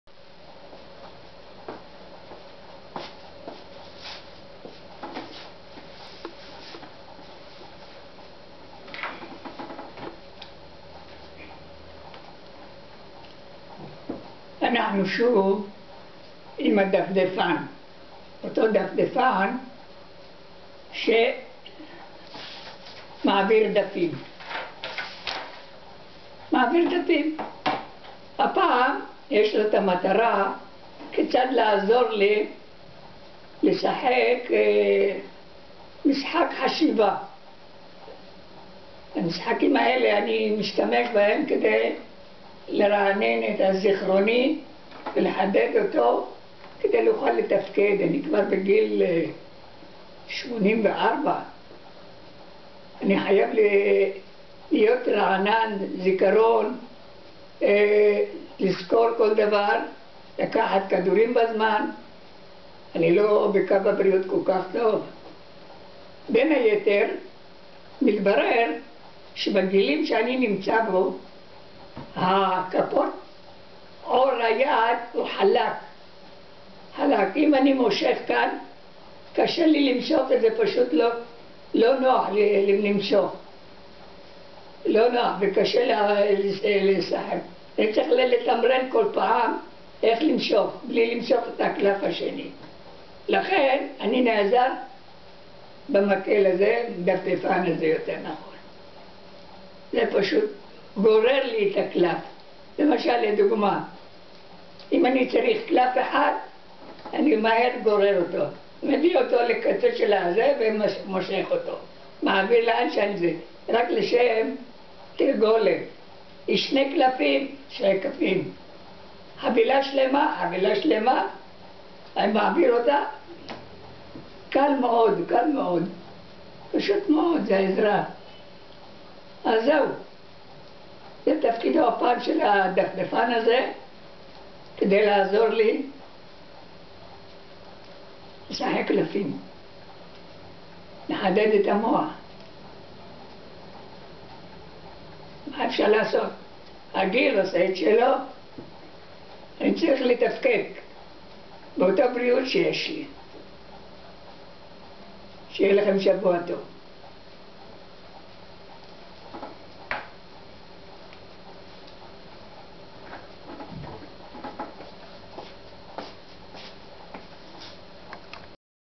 דפדפן לספרים וקלפים לעזרת אנשים הנמצאים במצוקה
אמצעי עזר לאנשים בבעלי מוגבלות מסוימת.
צילום רעיון משה רצון.